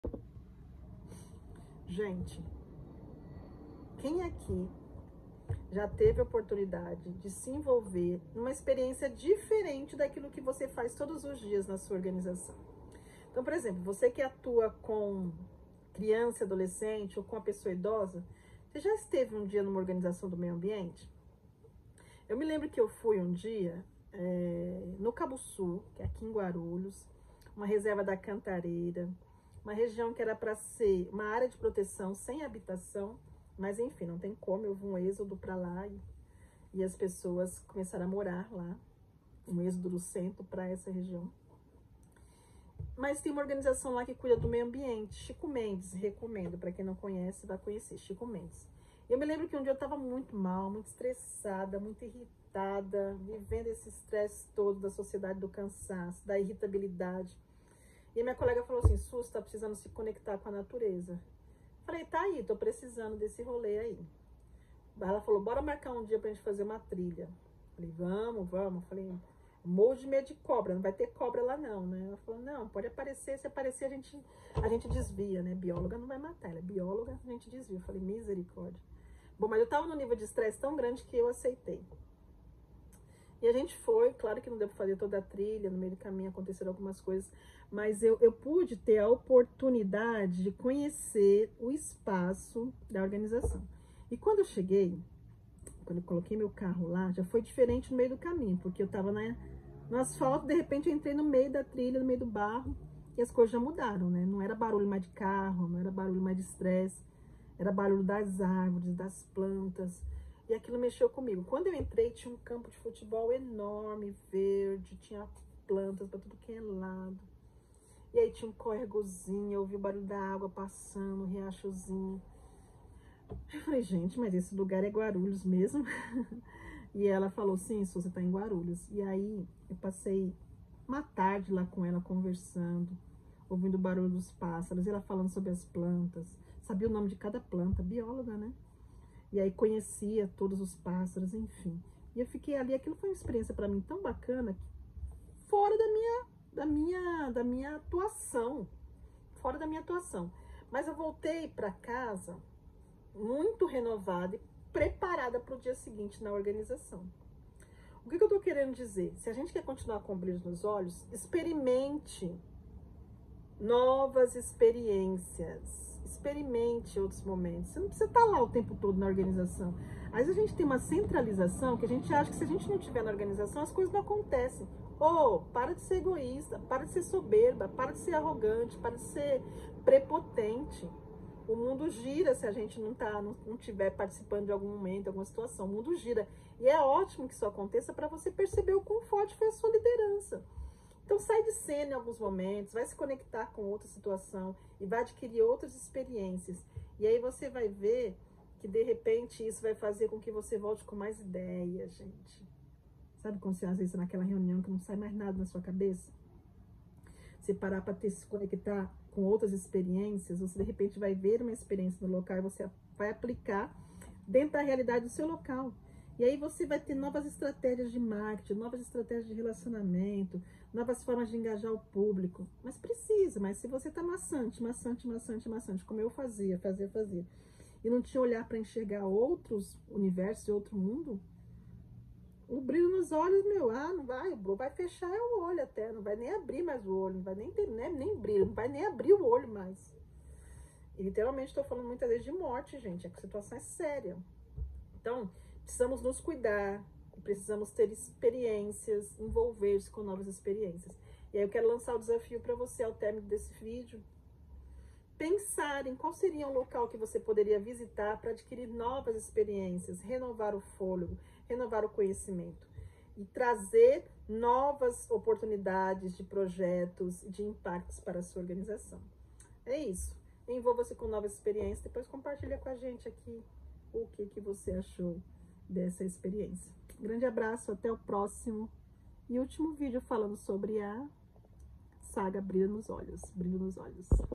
Saga: Brilho nos Olhos 🤩 - Experiências.
Envolva-se em novas experiências: Experiências diferentes estimulam a mente e trazem novas perspectivas. Em uma ONG, por exemplo, fazer um projeto inovador, experimentar uma nova estratégia de marketing ou ver uma nova forma de engajar o público pode trazer motivação renovada.